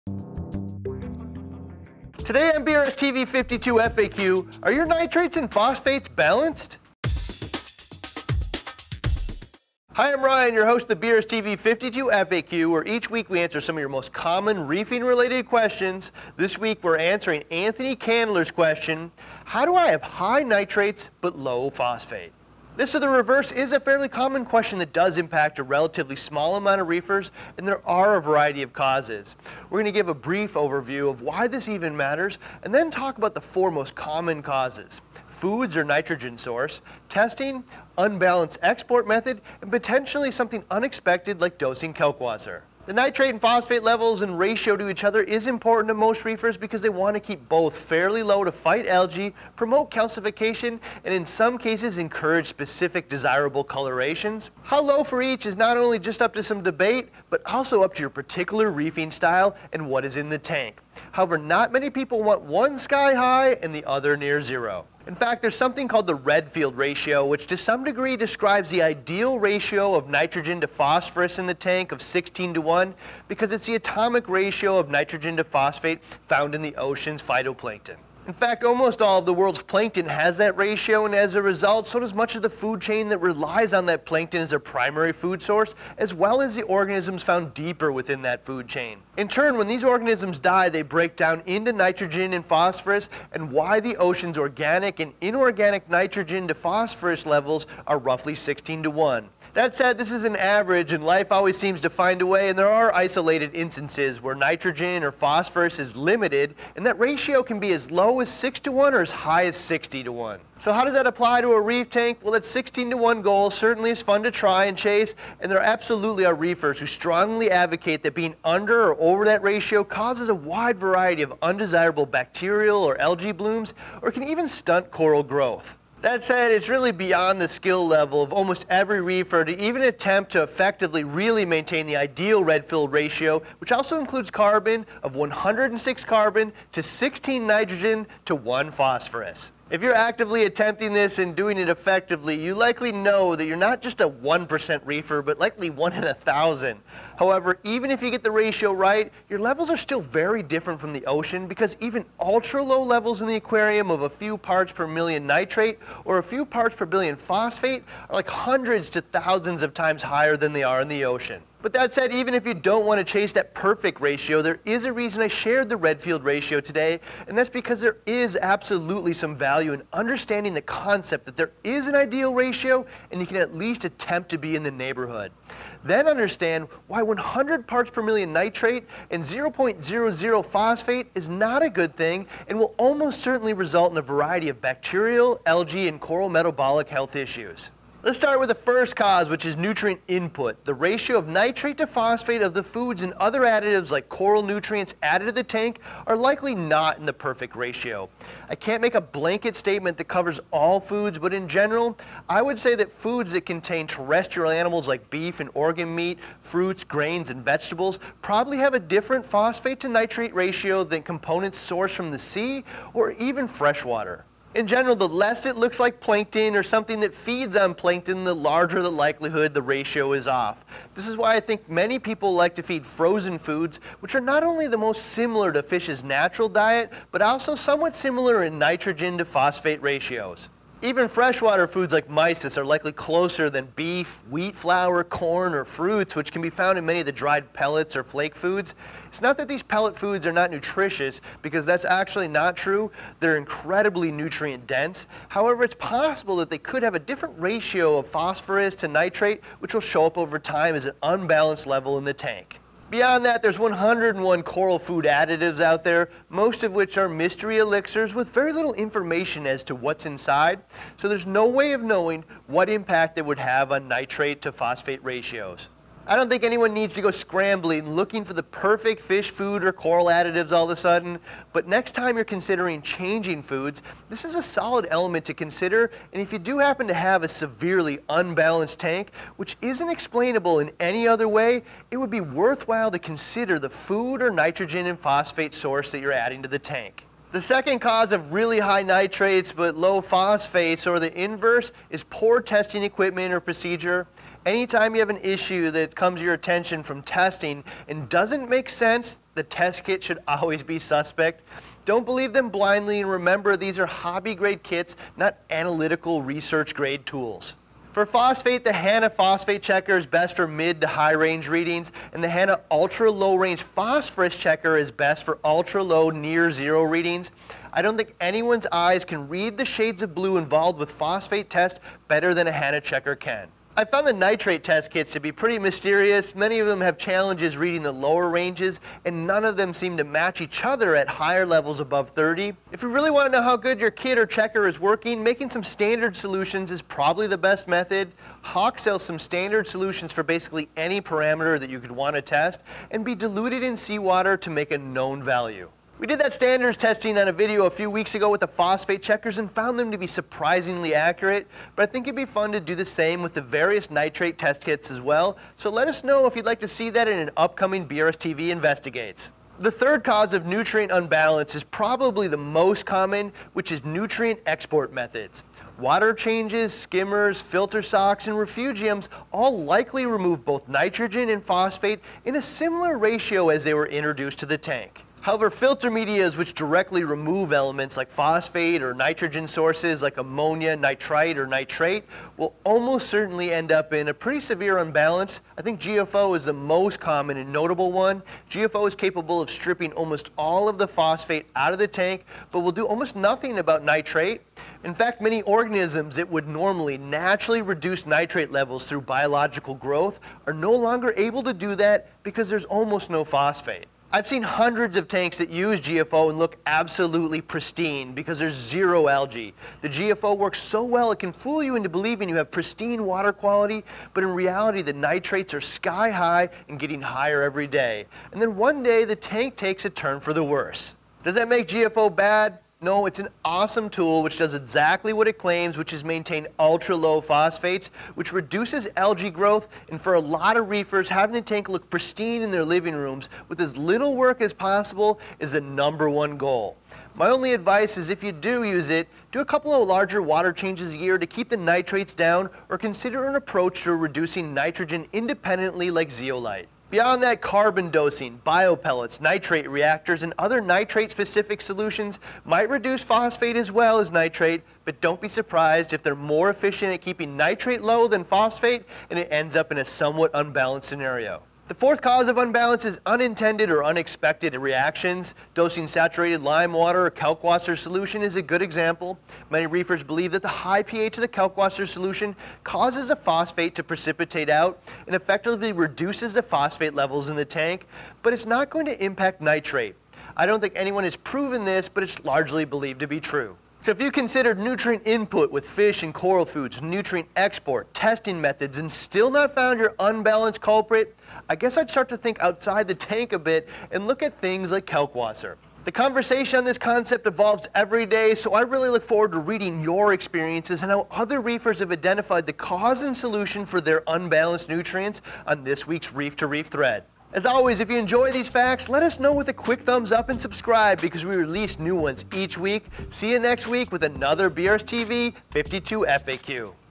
FAQ #42 Why are my nitrates high but phosphates low? | 52 FAQ
Today on BRStv, we have another episode of BRS 52 FAQ where we answer all of your frequently asked reefing questions from our popular 52 Weeks of Reefing series.

Today we are answering Anthony Candler’s question “How do I have high nitrates but low phosphate.?”

If you’ve ever had to fight high nutrients in your tank, you probably know how difficult it can be to bring them down. There are multiples of techniques to use that can be effective, but sometimes even those don’t seem to work and you’re left with an imbalance. Today, we discuss some causes to this imbalance and a talk about approaches to rebalancing them.

So sit down, hang out and watch as Ryan fixes his sights on today’s topic and shoots beams of reef knowledge your way, as well as provide you with additional ways to help make your reefing life a bit more fun and easy!